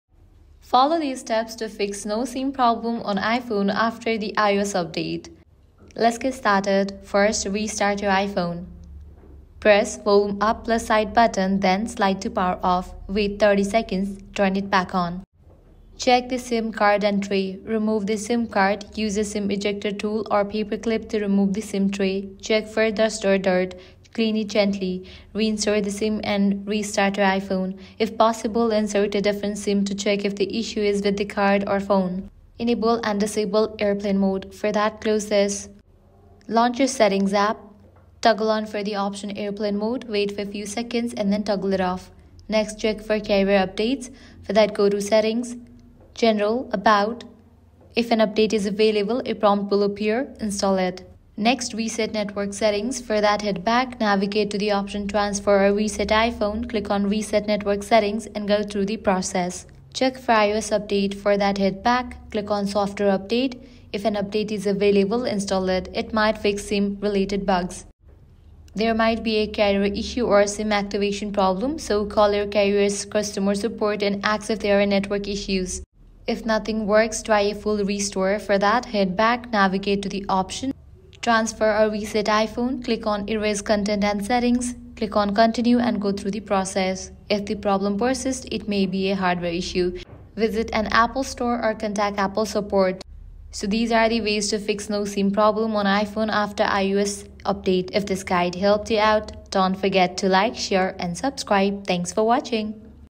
How to Fix NO SIM Problem on iPhone After iOS 18 Update
Experiencing a "No SIM" error on your iPhone after the iOS 18 update? In this video, we’ll guide you through the step-by-step process to fix the issue and get your SIM card working again. Whether it’s a software glitch or a network problem, this tutorial will help you resolve it quickly.

Timestamps:
00:00 – Why your iPhone shows "No SIM" after iOS 18 update
00:10 – Step-by-step guide to fixing the No SIM issue